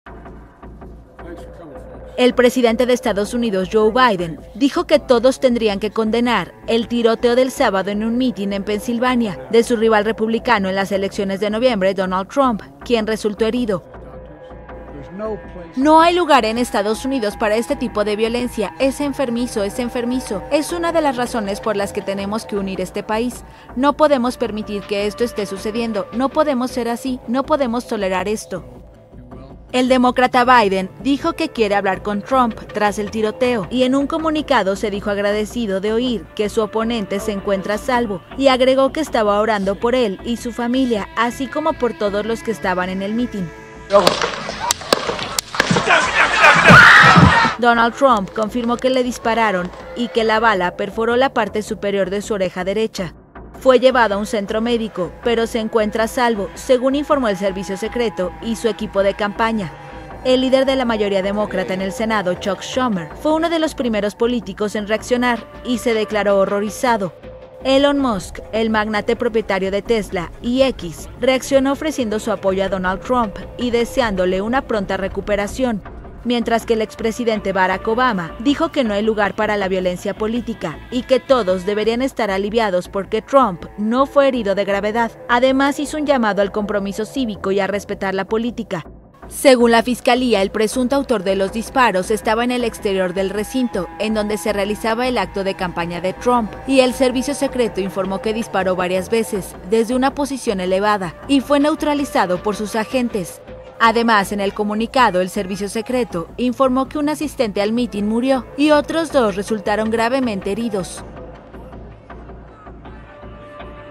BIDEN tras ATENTADO CONTRA TRUMP: "NO PODEMOS PERMITIRLO en ESTADOS UNIDOS" | Gestión
El presidente de EstadosUnidos, JoeBiden, dijo el sábado que todos deberían de condenar el tiroteo en el mitin de su rival republicano en las elecciones de noviembre, DonaldTrump, en el que el magnate resultó herido pero se encuentra “a salvo”.